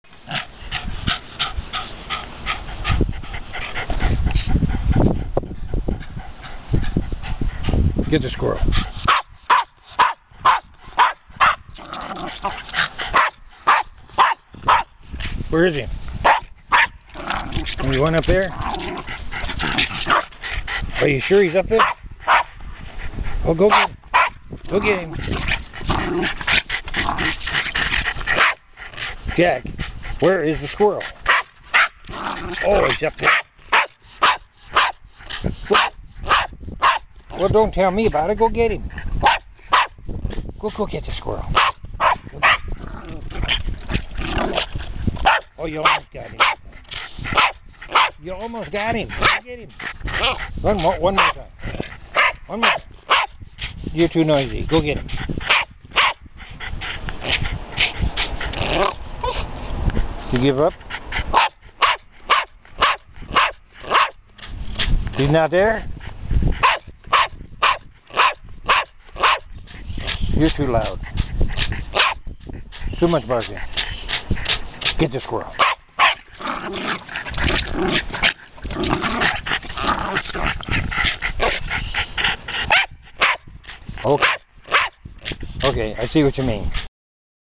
Jack the tree climbing dog
The squirrel may be long-gone .but it's not forgotten! Jack can not pass this tree without trying to get that squirrel.